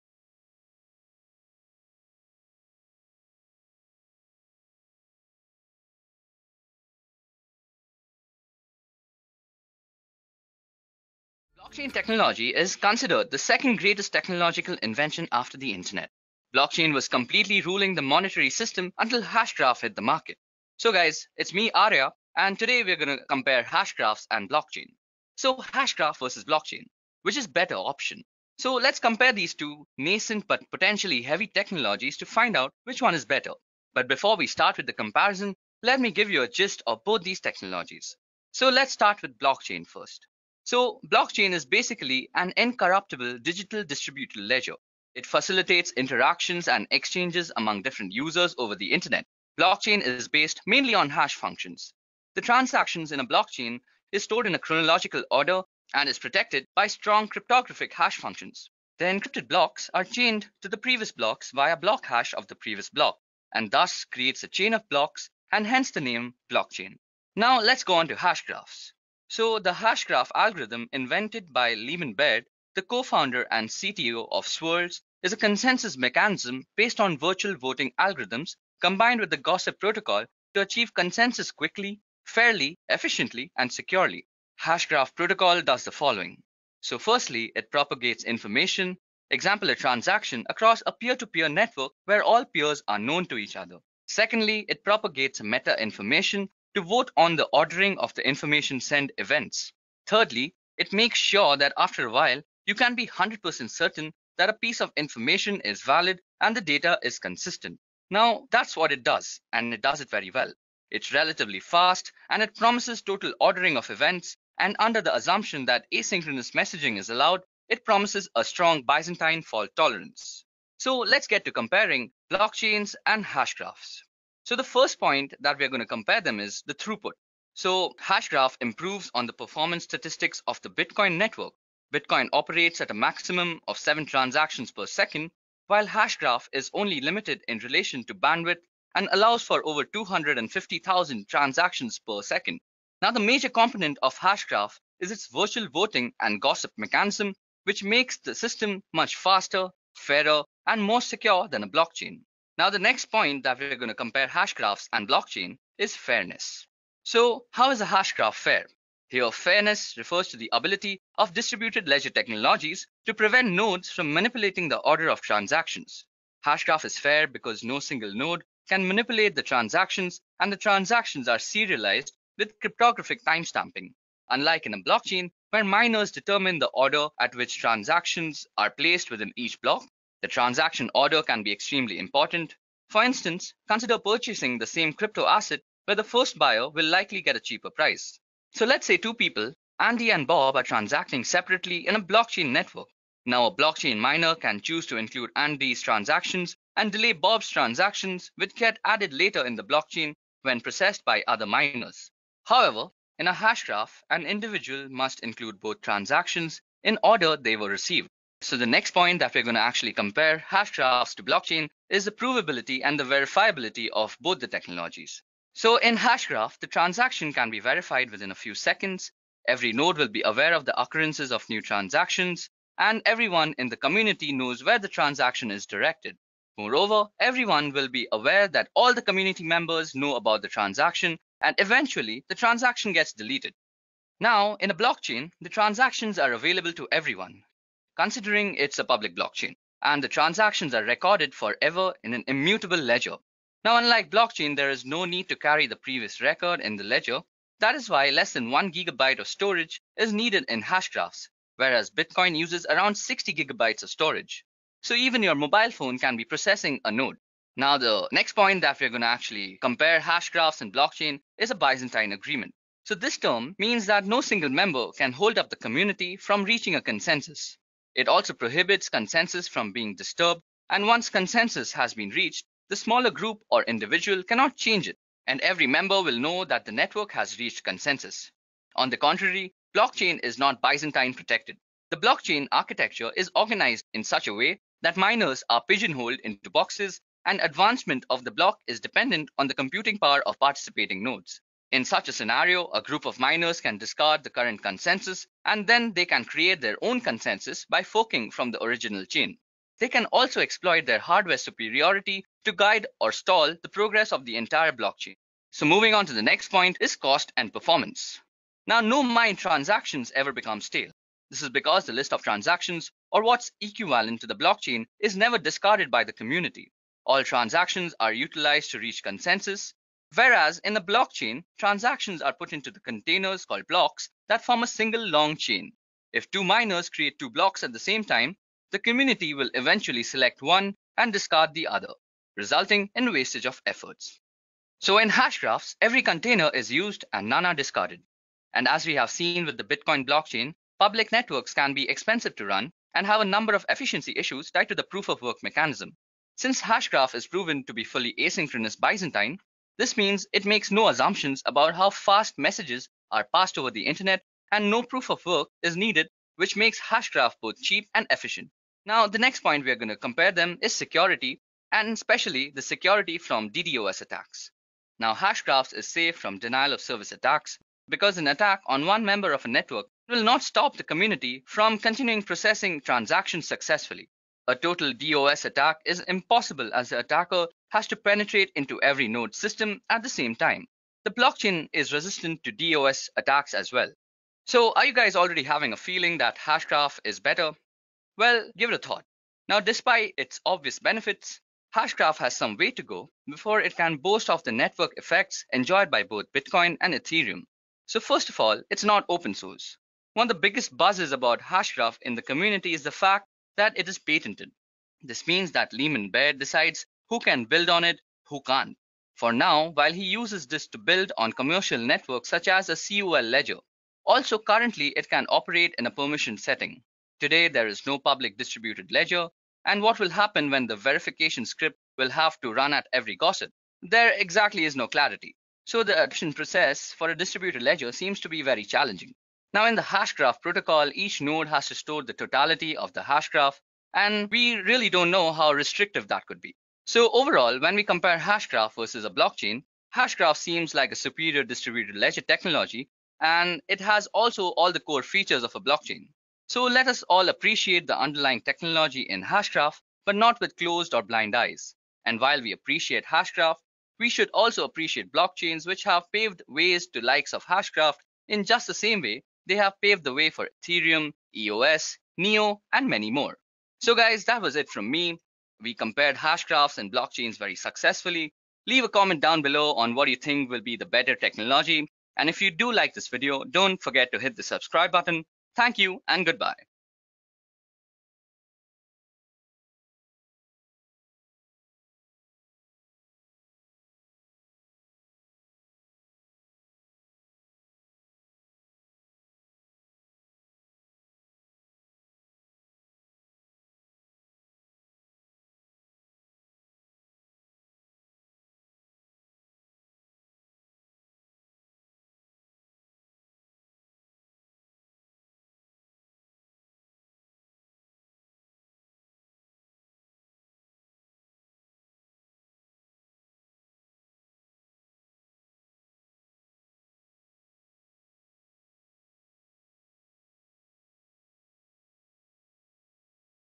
Hashgraph vs Blockchain | Hedera Hashgraph Tutorial | Hashgraph Technology | Edureka Rewind
Hashgraph is a consensus mechanism based on virtual voting. How is this better than Blockchain? or Is it? Find out while we compare these two in this short video.